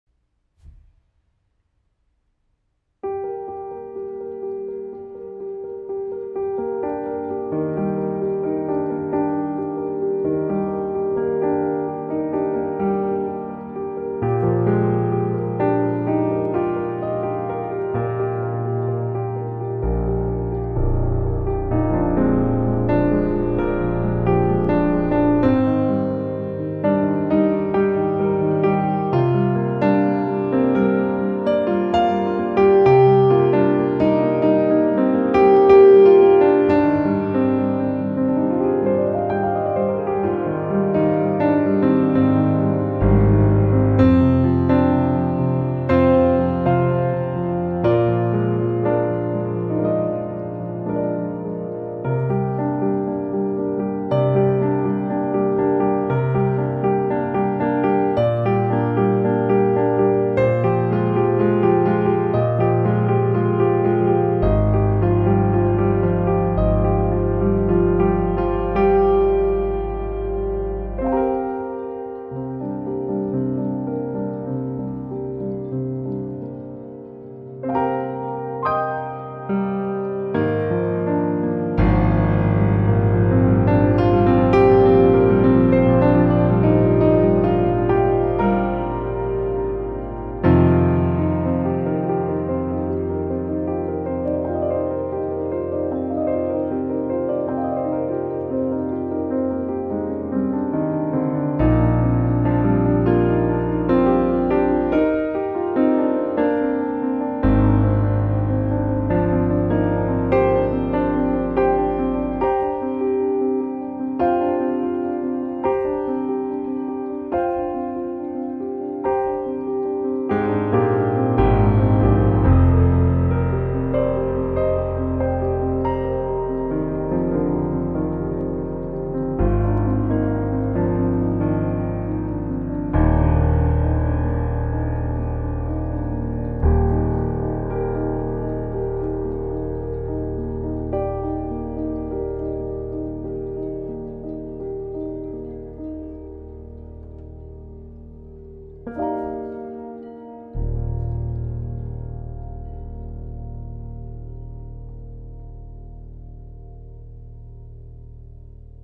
Pianography - Pianograph #4
For a while, I'm developping a setup where I can play the piano and it's writing in live, in Houdini, what i'm playing, in this kind of calligraphic way.
The fun part about that, it's that the way I'm playing is driven by what I'm seeing on the screen; so it creates very interesting improvisations.